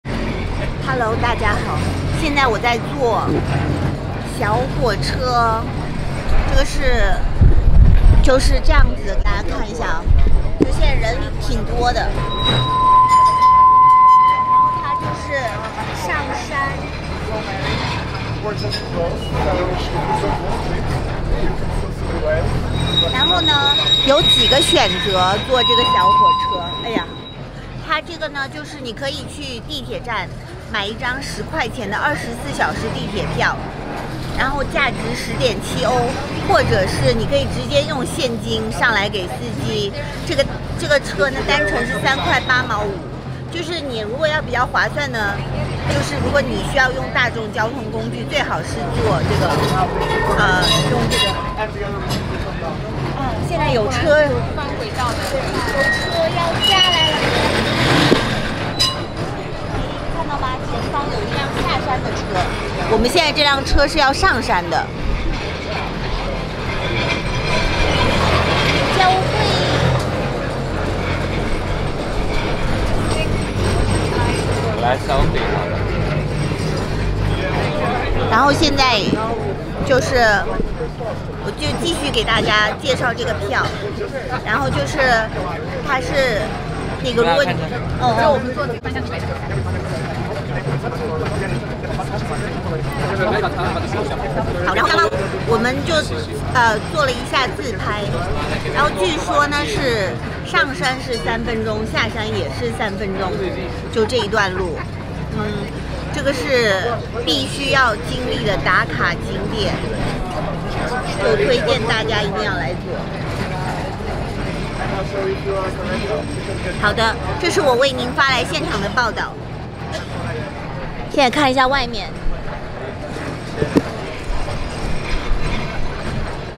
Travel with Renee | Cable car tour at Lisbon | 和我一起坐里斯本小火车
Different ticket options to take cable car at downtown Lisbon